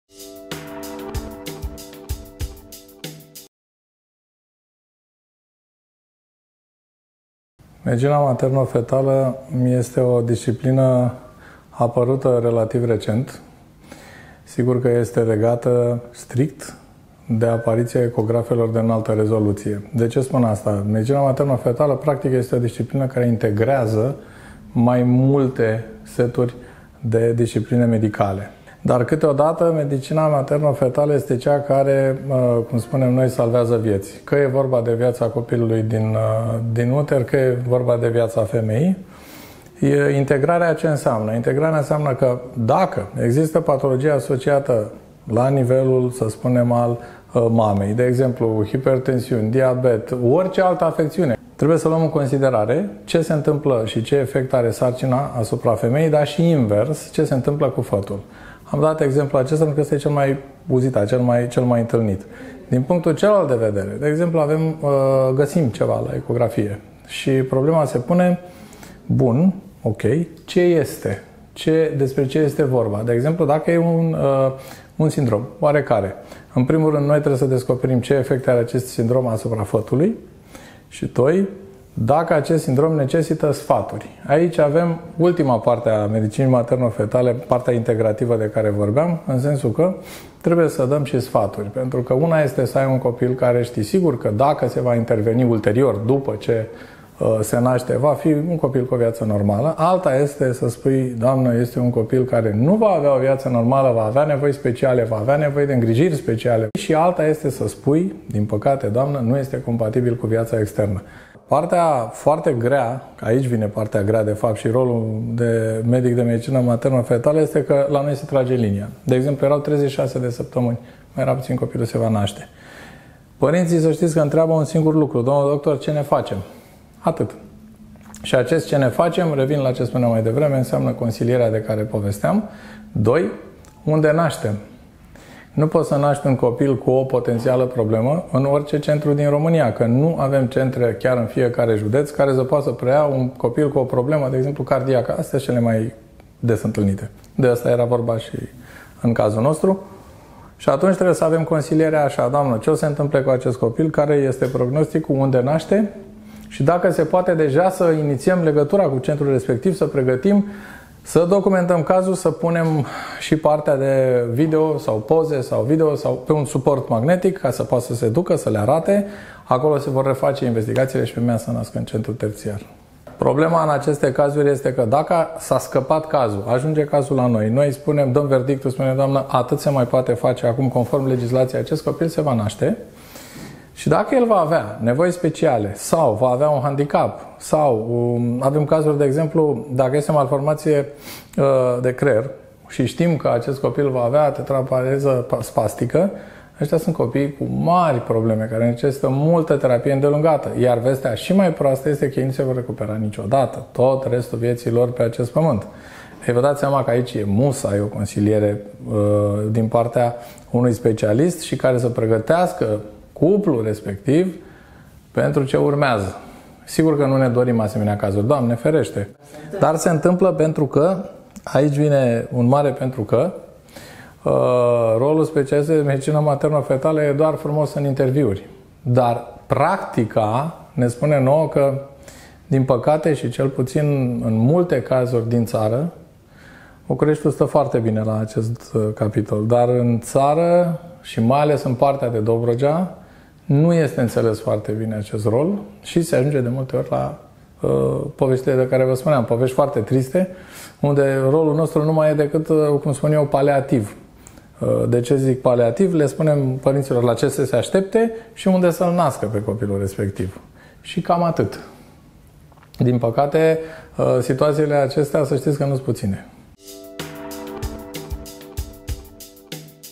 Rolul specialistului de medicină materno-fetală în zilele noastre
Interviu cu Dr. Răzvan Stanca medic primar obstetrică ginecologie  specialist morfolofie fetală Hyperclinica MedLife Constanța